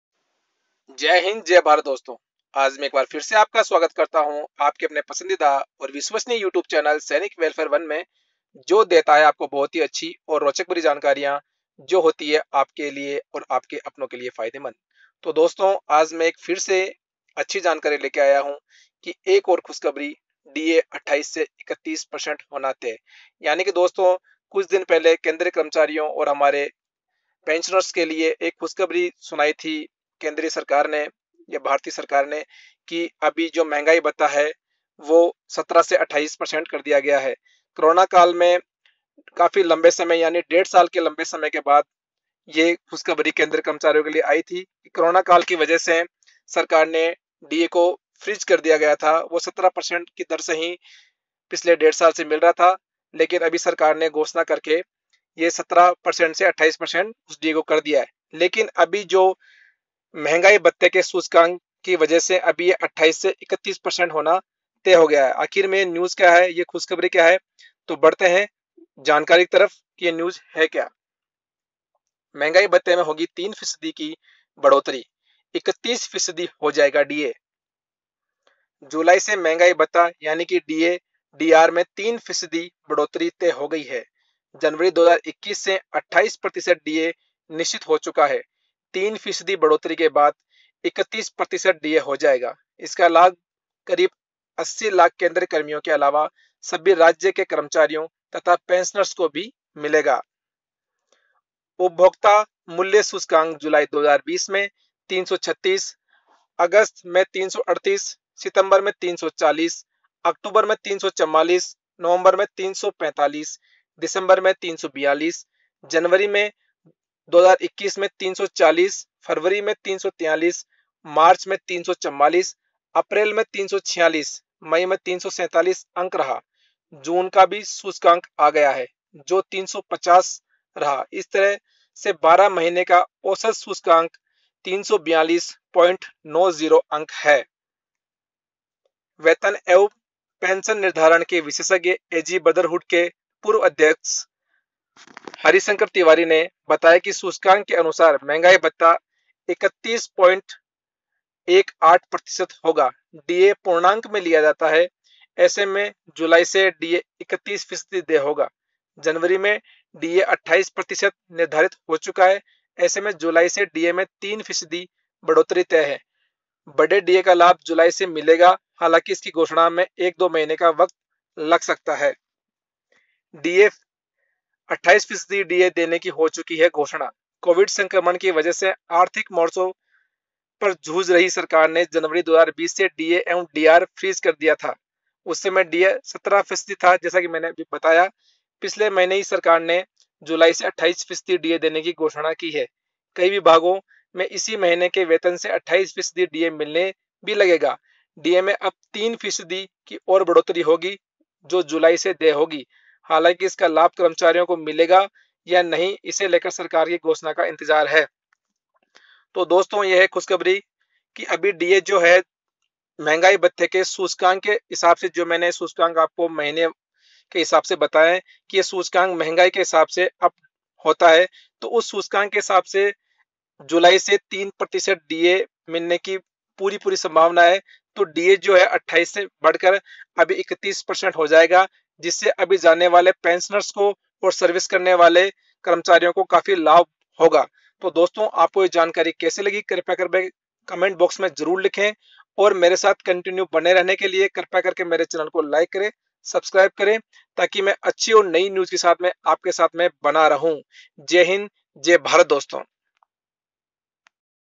खुसखबरी महंगाई भत्ता हुवा 28% से 31%
दोस्तों मैंने इस पोस्ट के अंदर महंगाई भत्ते के बारे में जानकारी दी है कि महंगाई भत्ता किस प्रकार बढ़ता है और जो सरकार ने अभी महंगाई भत्ता करो ना काल में फ्रिज  कर दिया गया था उसको अनफ्रीज करने के बाद में महंगाई भत्ता जुलाई में भी 3% बढ़ा दिया गया है उसके बारे में कंप्लीट जानकारी मैंने दी है